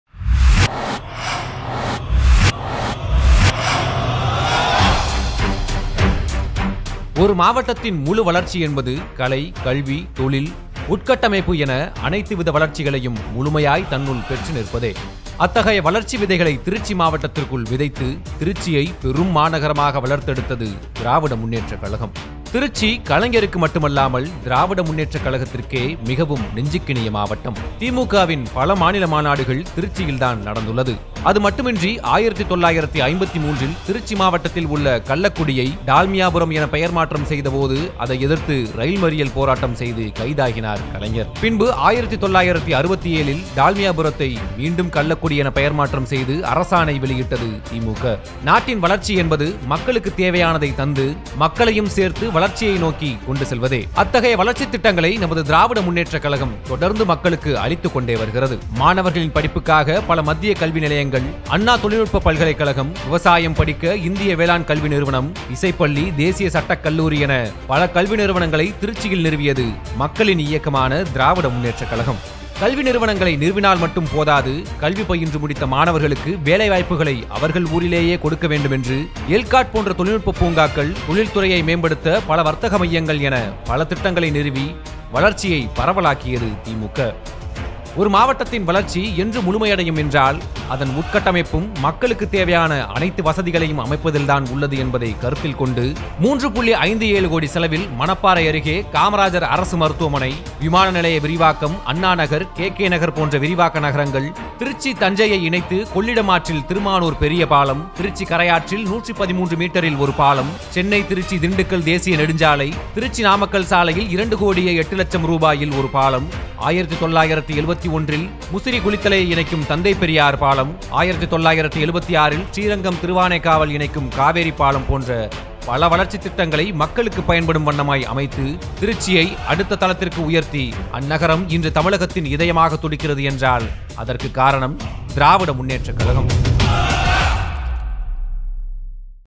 திமுகவின் மகத்தான மாவட்ட சாதனைகள் | திருச்சி மாவட்டம்
உள்கட்டமைப்பு வசதிகள் பெருமளவில் செய்து கொடுத்து மாவட்ட வளர்ச்சிக்கு வித்திட்ட இயக்கம் திராவிட முன்னேற்றக் கழகம்.